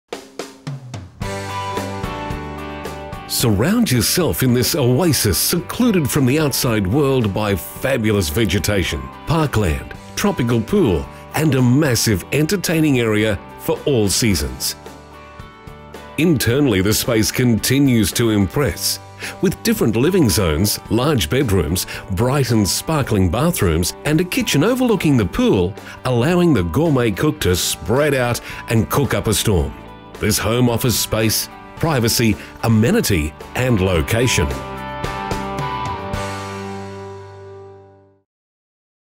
37 Sutherland Drive, Buderim QLD By LJ Hooker Mooloolaba
Property Video shoot of 37 Sutherland Drive, Buderim QLD by PlatinumHD for LJ Hooker Mooloolaba